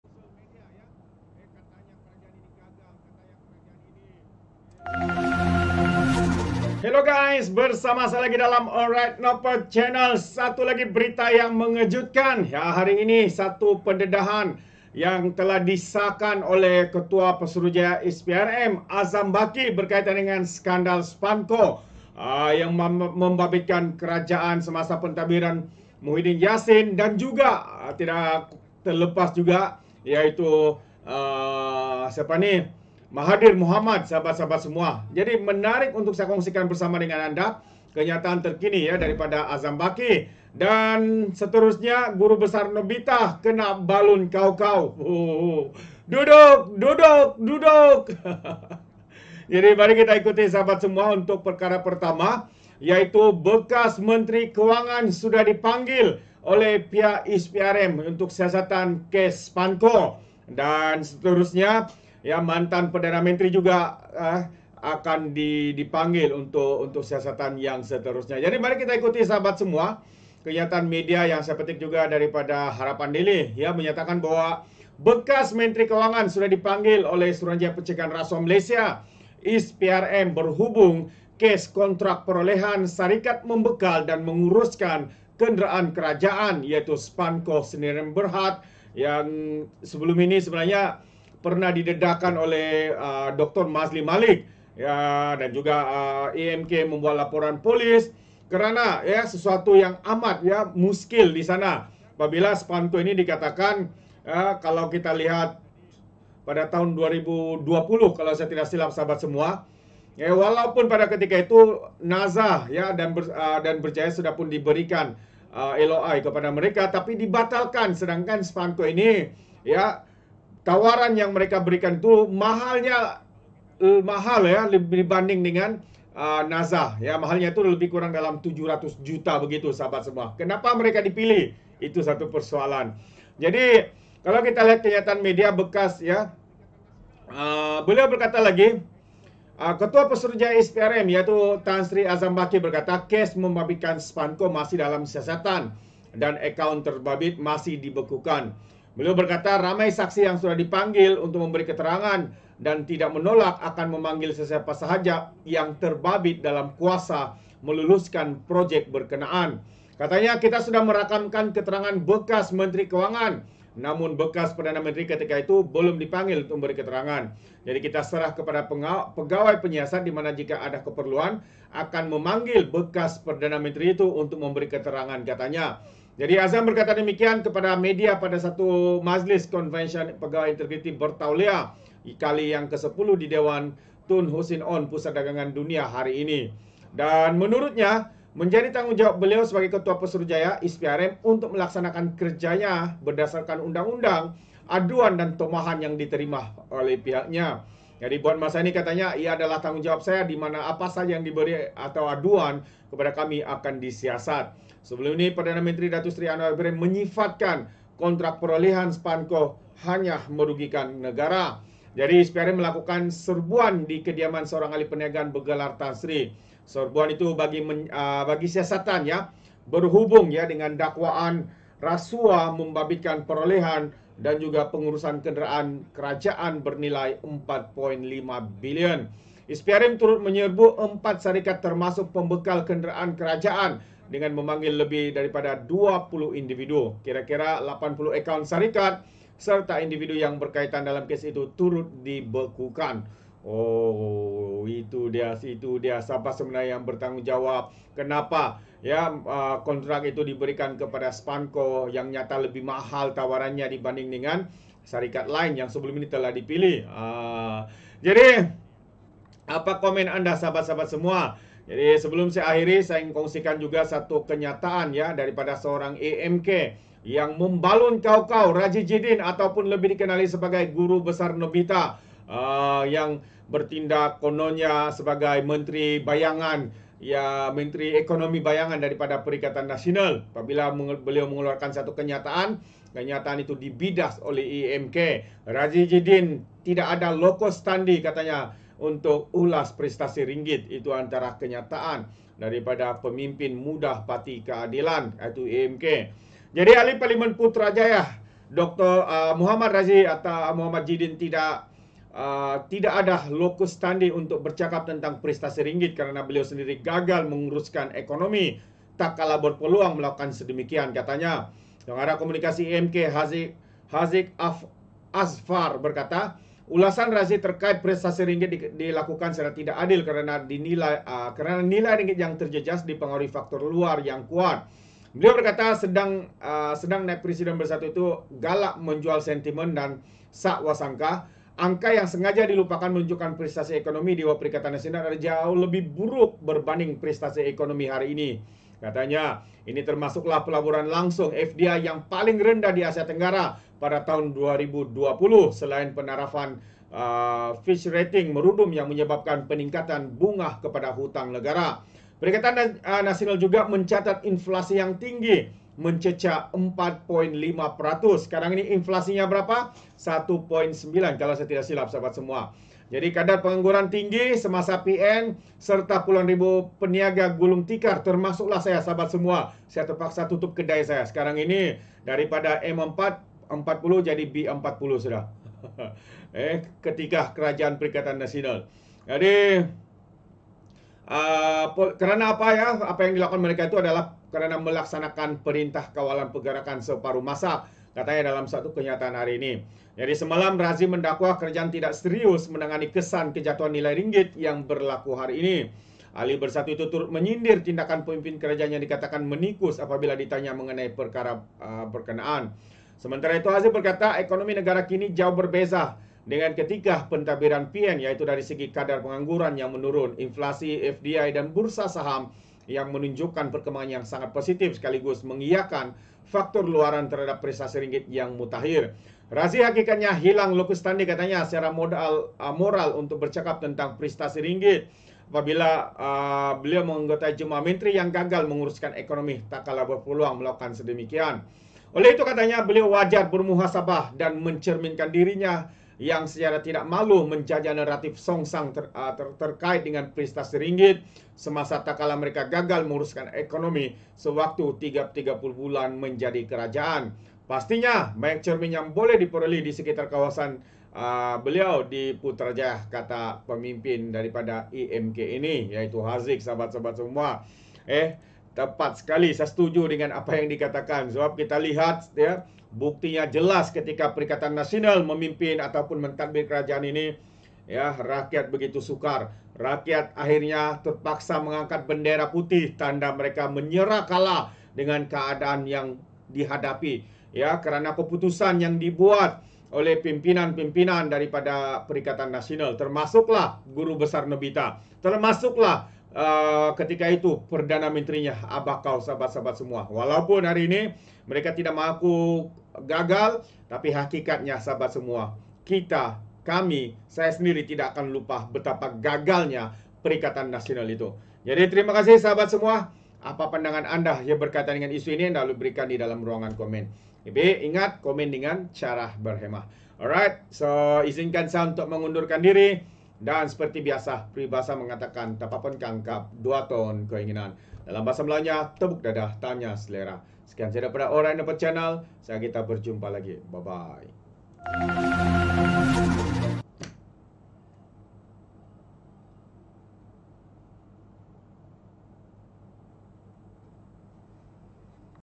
PENDEDAHAN SKANDAL SPANCO! GURU BESAR NOBITA KENA BALUN KAW KAW!
Bekas menteri kewangan sudah dipanggil Suruhanjaya Pencegahan Rasuah Malaysia (SPRM) berhubung kes kontrak perolehan syarikat membekal dan menguruskan kenderaan kerajaan, Spanco Sdn Bhd (Spanco).
Ketua Pesuruhjaya SPRM, Tan Sri Azam Baki berkata, kes membabitkan Spanco masih dalam siasatan dan akaun terbabit masih dibekukan.
Beliau berkata, ramai saksi yang sudah dipanggil memberi keterangan dan tidak menolak akan memanggil sesiapa sahaja yang terbabit dalam kuasa meluluskan projek berkenaan.
“Kita sudah merakam keterangan bekas menteri kewangan namun bekas Perdana Menteri ketika itu belum dipanggil memberi keterangan.

“Kita serah kepada pegawai penyiasat di mana jika ada keperluan akan memanggil bekas Perdana Menteri itu untuk memberi keterangan,” katanya.

Azam berkata demikian kepada media pada Majlis Konvokesyen Pegawai Integriti Bertauliah (CEIO) kali ke-10 di Dewan Tun Hussein Onn, Pusat Dagangan Dunia (WTC), hari ini.